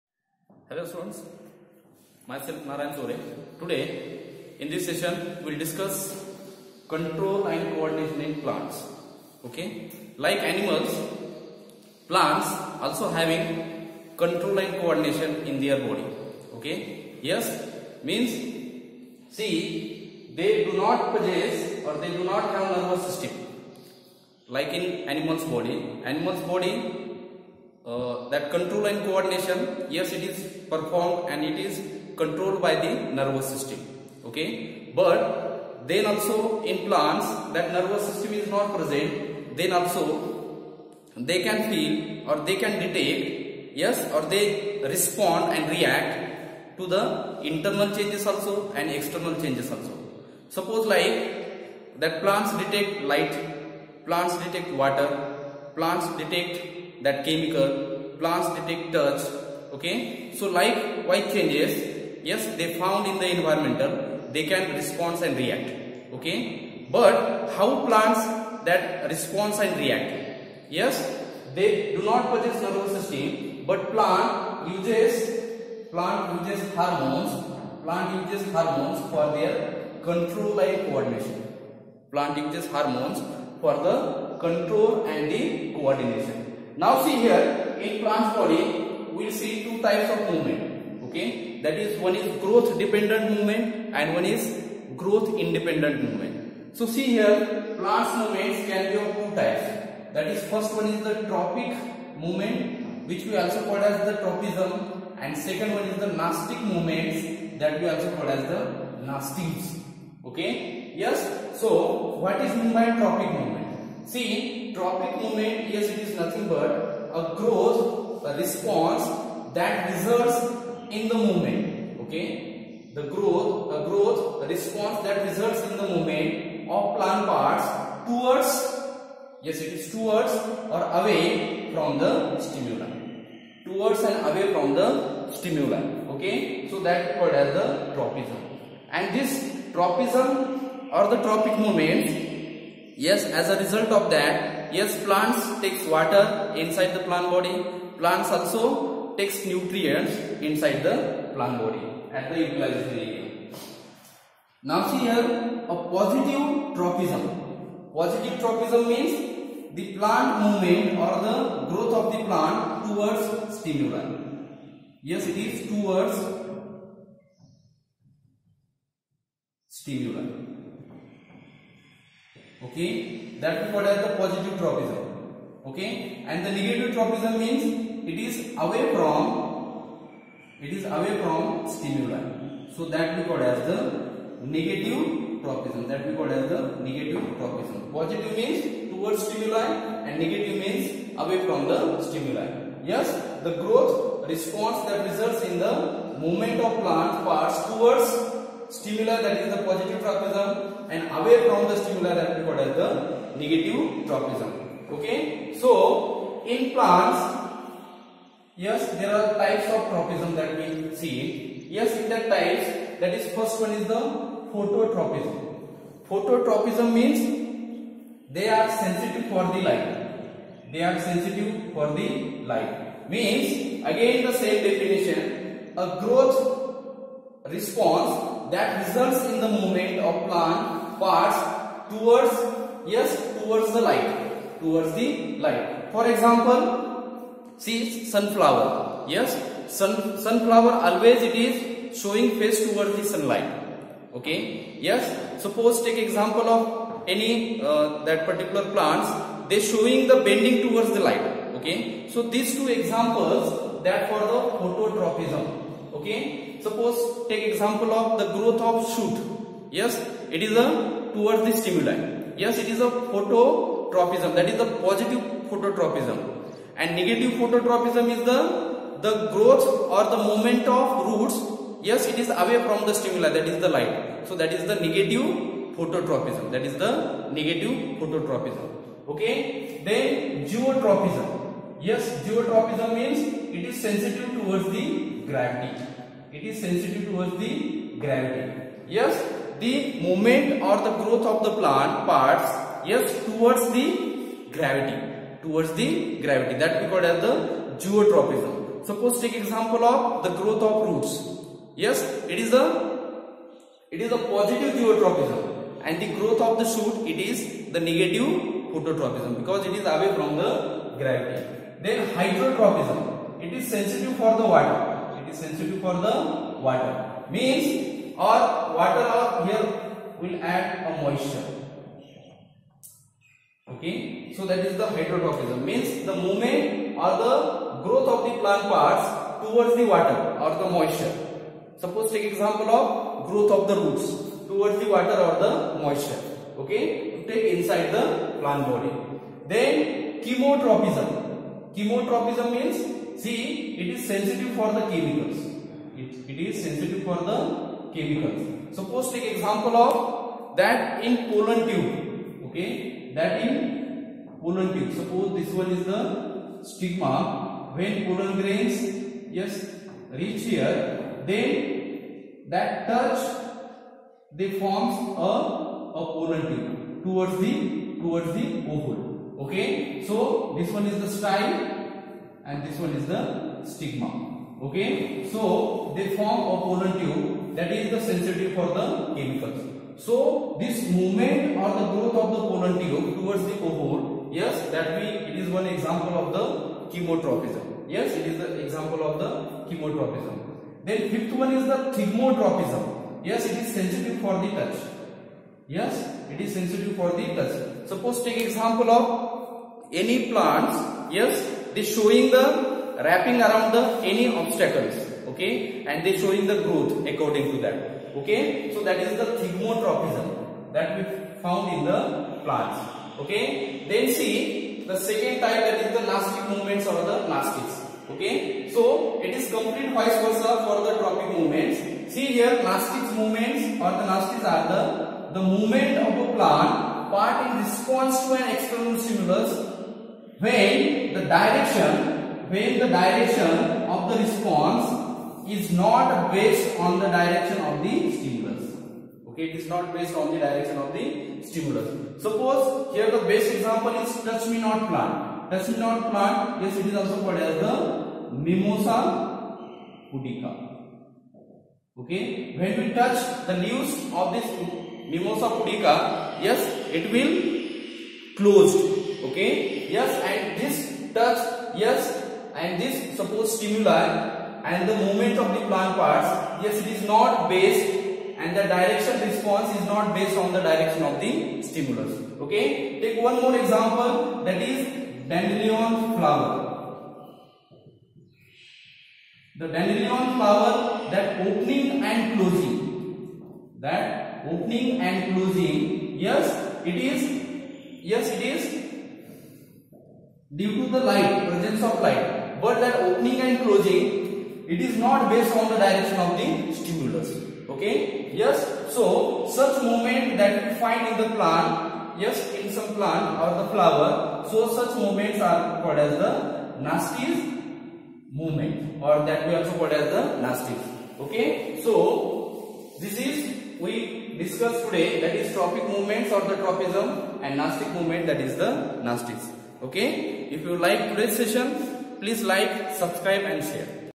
Control and Coordination| tropic movements|Nasties|Tropism types|Class 10
Biology live class by chaure nb